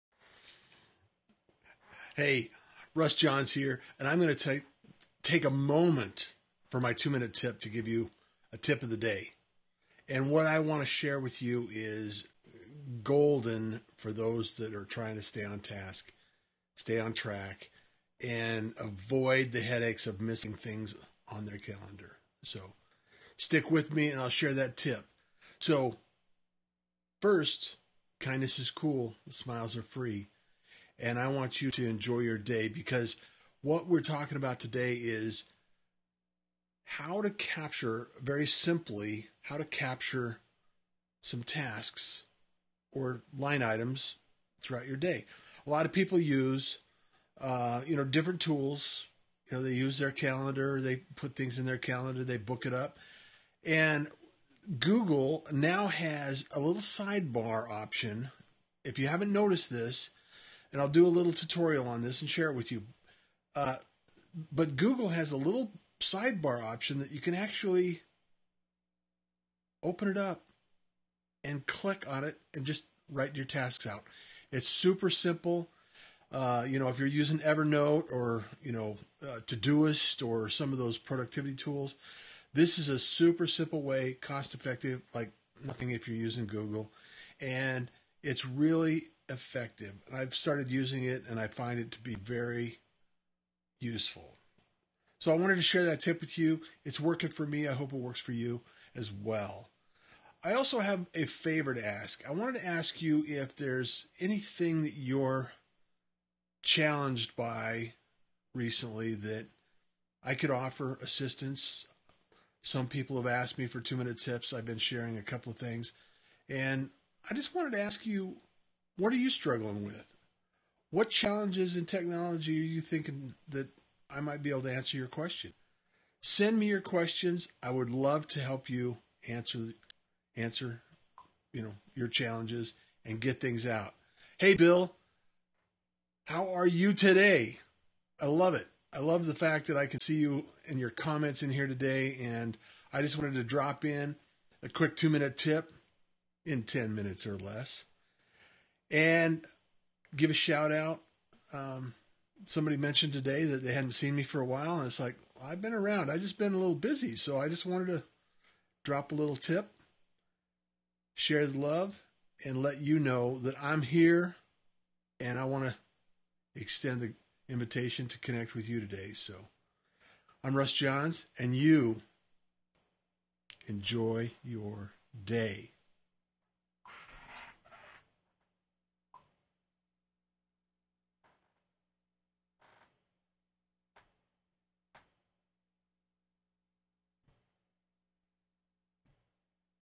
How to capture quick ideas with Gmail.
Learn how you can capture quick ideas withoutleaving Gmail.